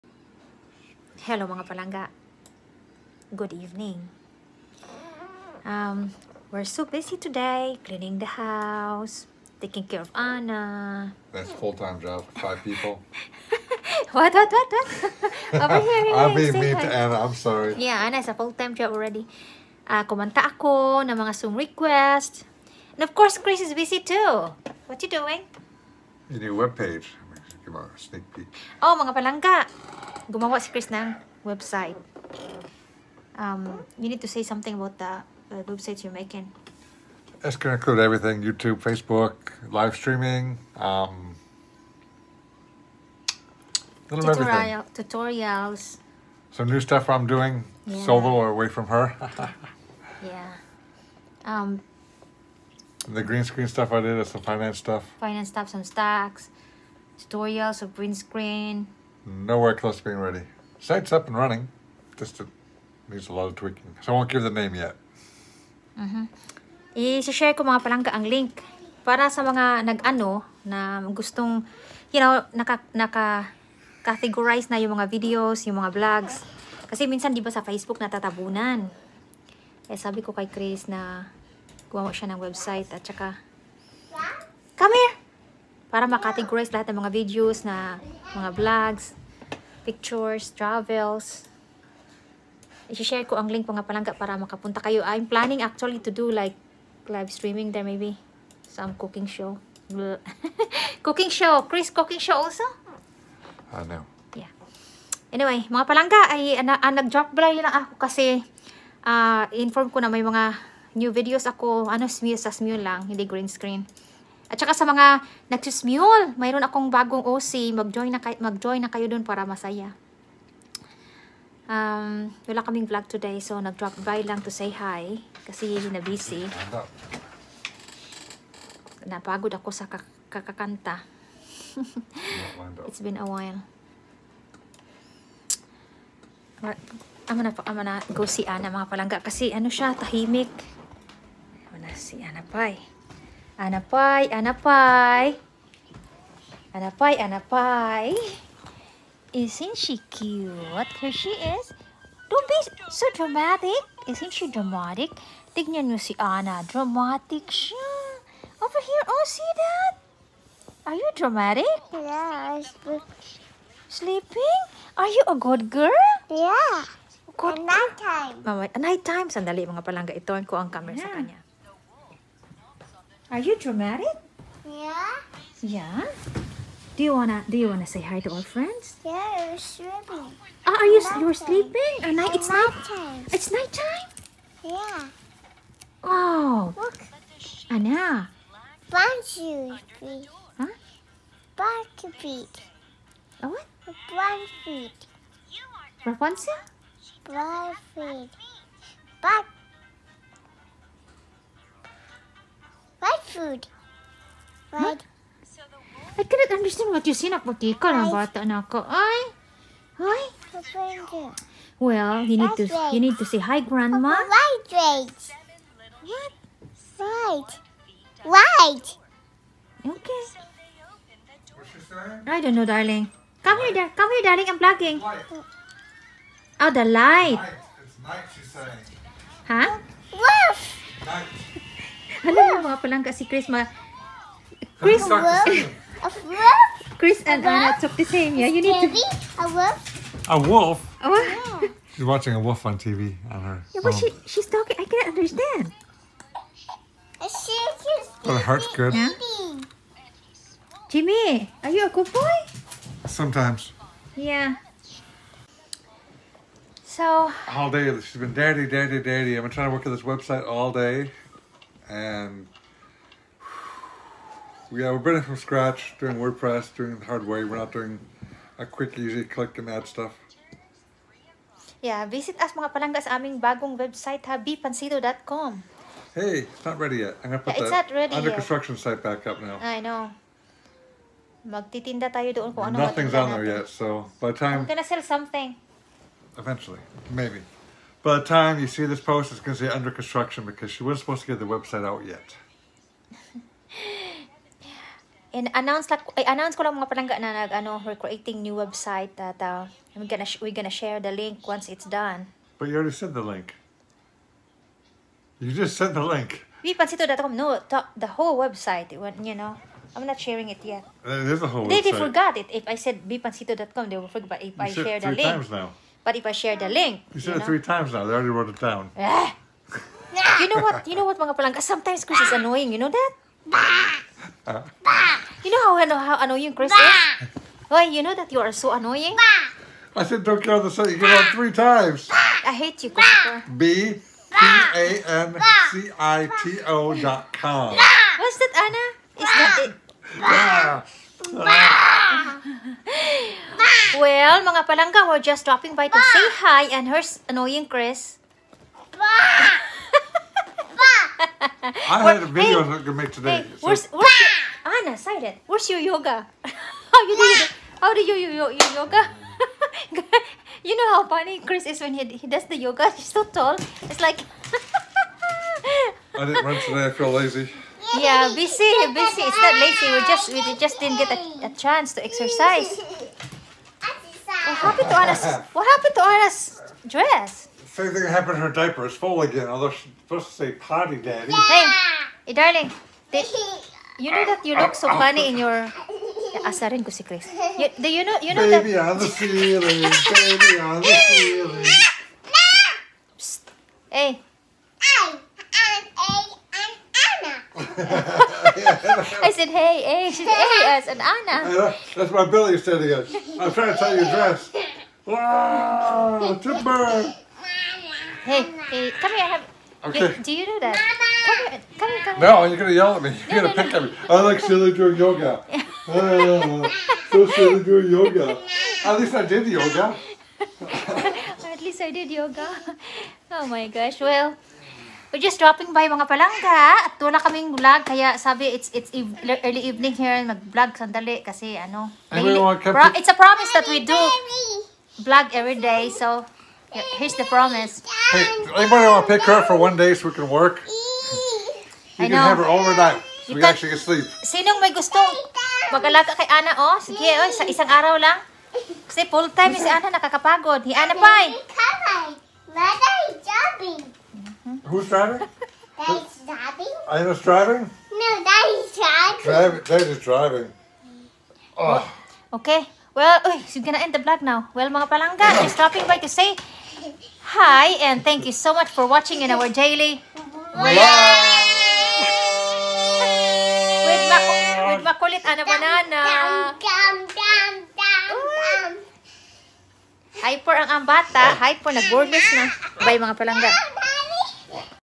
Busy Day To Tease Chris
We’re making new website today and for fun I tease Chris. Lol.